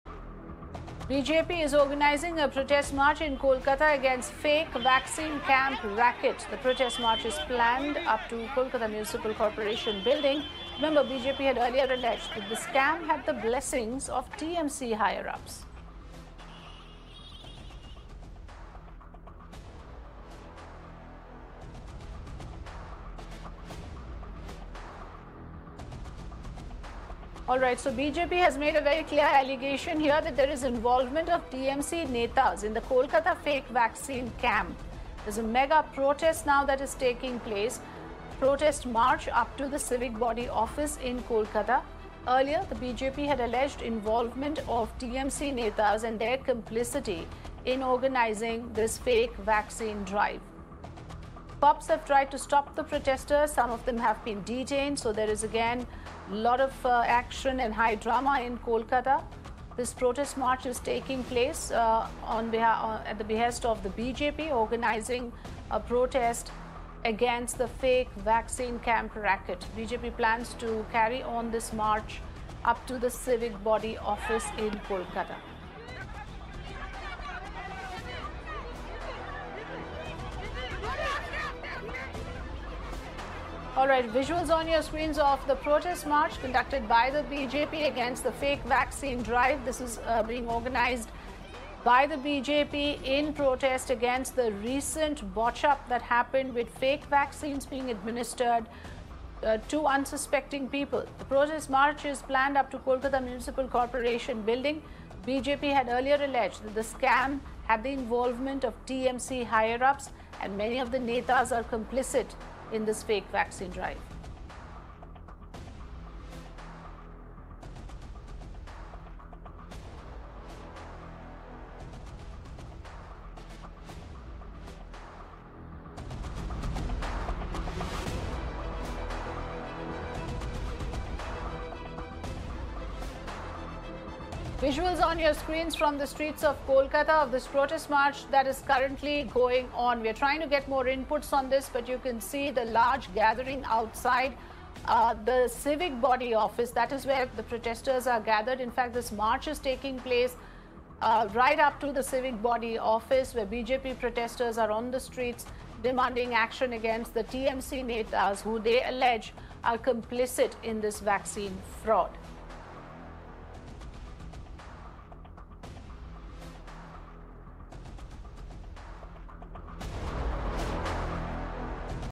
West Bengal: BJP protests against fake vaccine camp racket in Kolkata
BJP is staging a protest march in Kolkata against a fake vaccine camp racket. The protest march is planned till Kolkata Municipal corporation building. Remember, BJP has earlier alleged the involvement of Trinamool Congress leaders in the fake vaccine scam. Police have tried to stop protesters and some of them are detained. Meanwhile, clashes erupted between the BJP activists and the police. BJP State party chief Dilip Ghosh attacked the West Bengal government saying that the law and order situation has deteriorated in the state. the party is demanding action against Trinamool Congress leaders who they alleged are complicit in this fake vaccine scam racket.